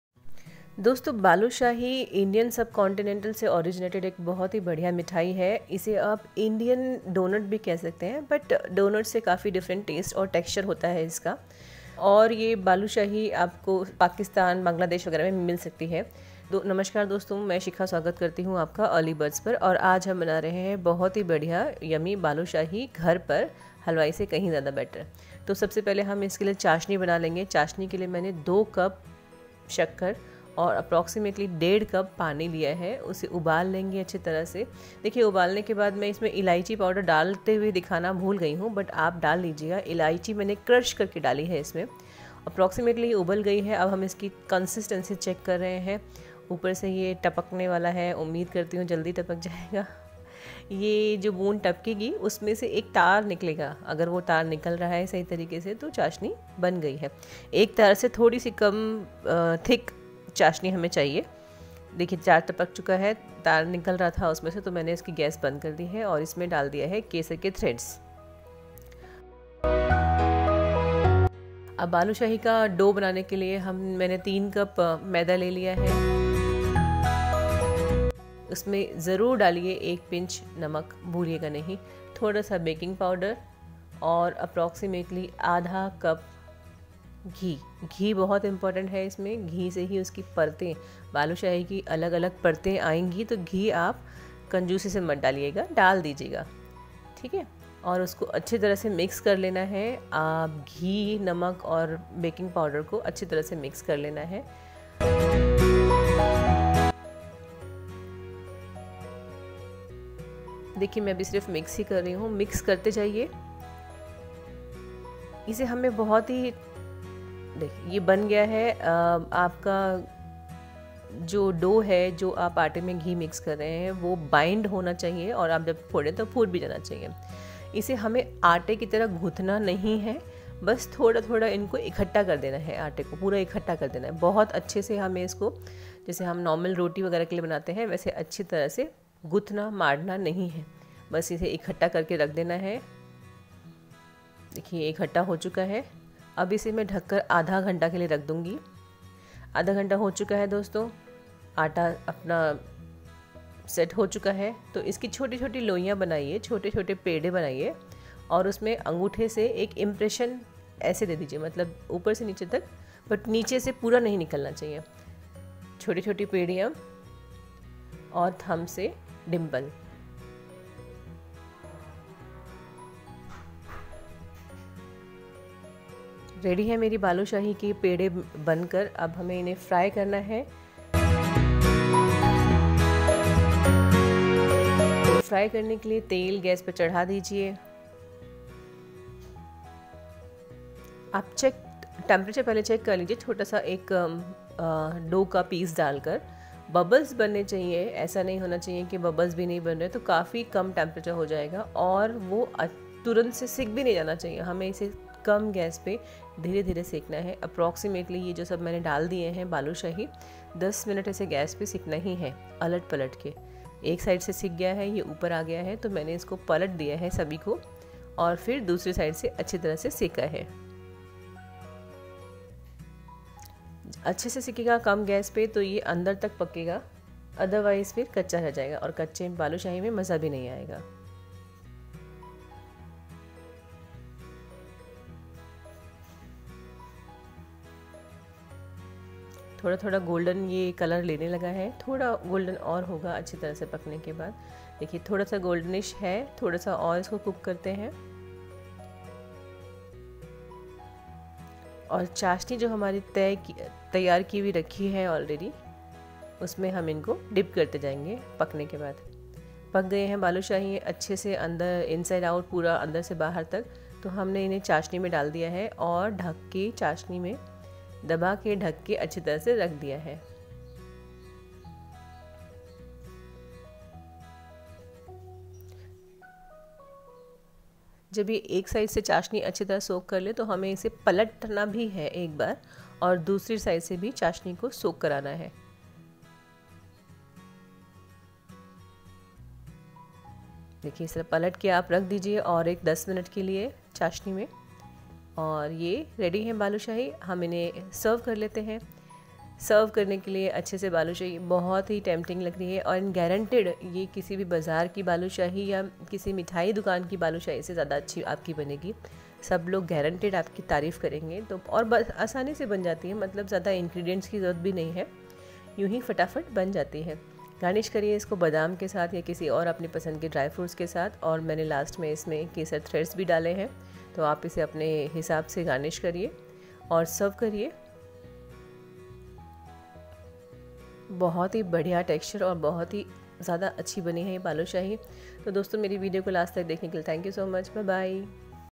balushahi, Halwai Jaisi Balushahi | सिर्फ 3 चीजों से बालूशाही | खास तरीके से रसीली व खस्ता बनाये
Balushahi is a traditional dessert originating from the Indian subcontinent. It is similar to a glazed doughnut in terms of ingredients, but differs in texture and taste. In South India, a similar pastry is known as badushah.

INGREDIENTS:
For Dough:
* All-purpose flour (मैदा) - 2.5 cup/350 grams
* Salt (नमक) - 1 pinch
* Baking powder (बेकिंग पाउडर) - 1 tsp
* Ghee (घी) - 1/2 cup/100 grams
* Water (पानी) - less than 1/2 cup
* Oil/Ghee (तेल/घी) - for frying
* Dry fruits (सूखे मेवे) - some

For Sugar Syrup:
* Sugar (चीनी) - 2 cup/400 grams
* Cardamom powder (इलायची) - 2-3
* Saffron (केसर)